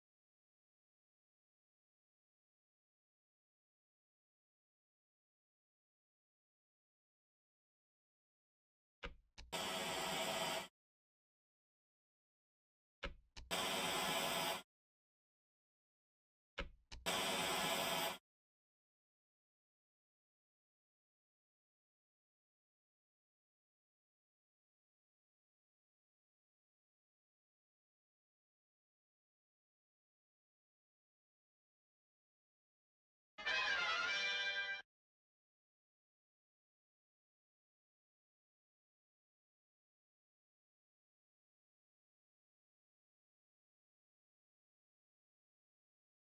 What I have so far - IGMC2018
Working on my entry for the game jaaaaaam. You are an old guy who murders other old people to increase your monthly Medicare payout. Dark? Maybe. Funny? Yes.